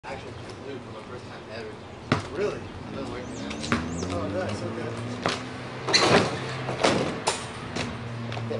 lewis dunks
lewis throws it down at phi delt